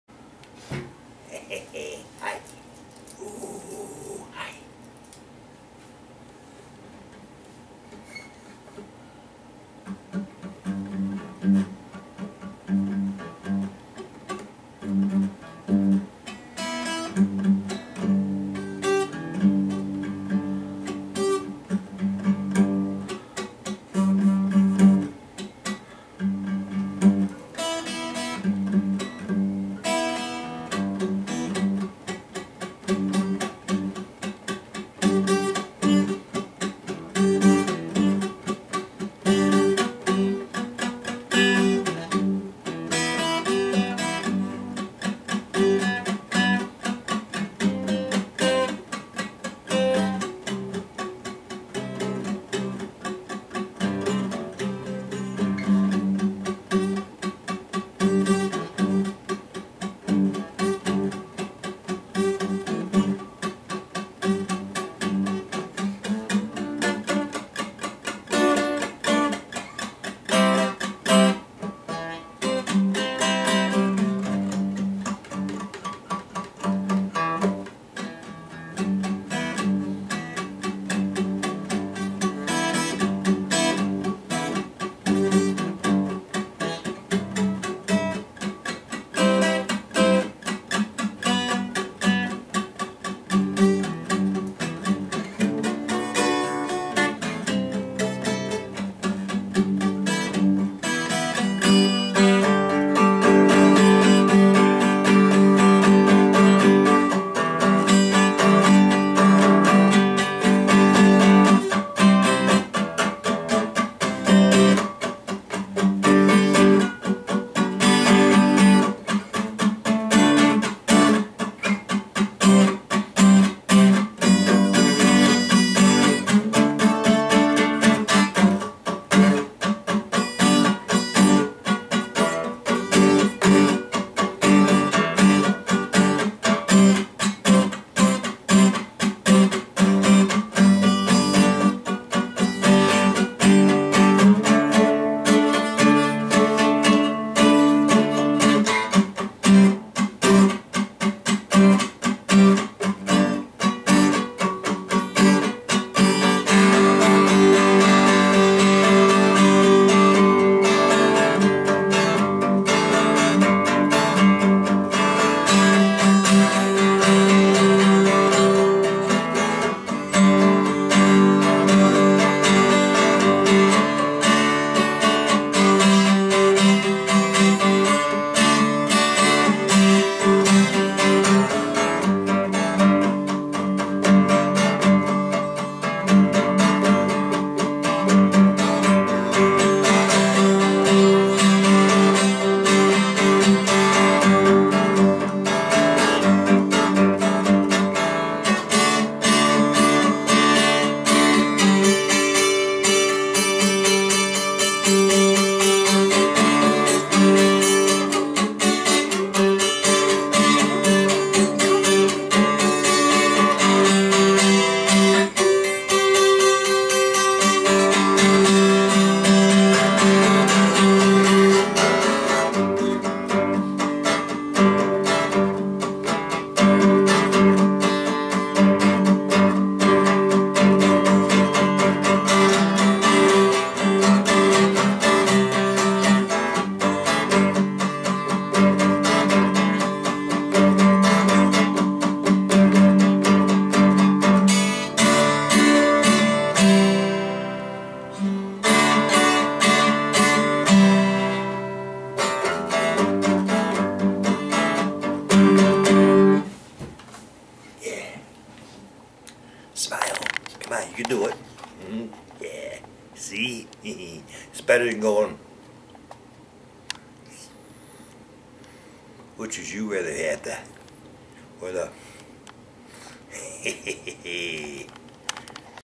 little guitar work 1
just a little guitar work on my 12 string   lets have some fun,,dance if you want to  yea